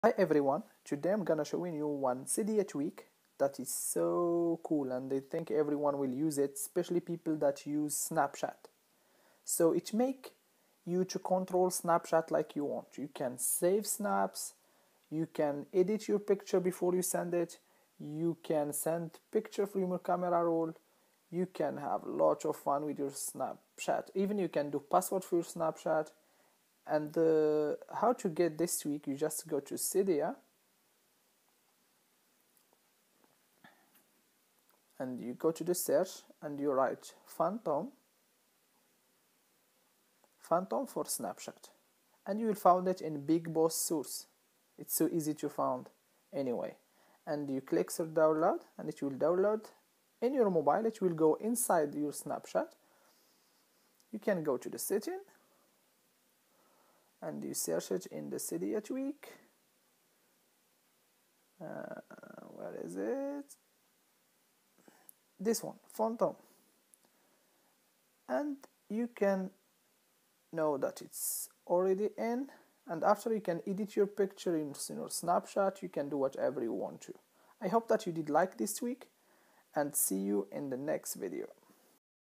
Top Cydia tweak phantom for snapchat for all ios devices
How to hack snapchat with This amazing phantom for snapchat tweak in bigboss source for cydia iphone and iPad and all ios devices